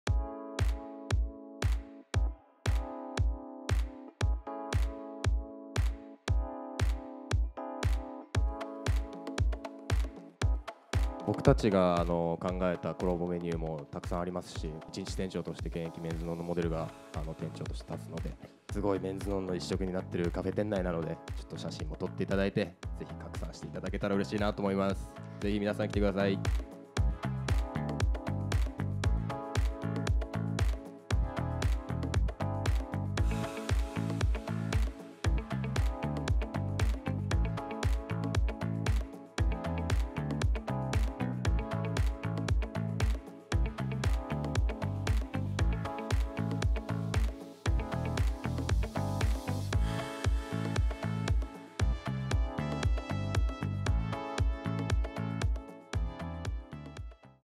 メンズノンノカフェ紹介映像
一週間限定のコラボカフェ、オープン！
店の雰囲気とコラボメニュー、発表会でのメンズノンノモデルのコメントを動画にまとめました。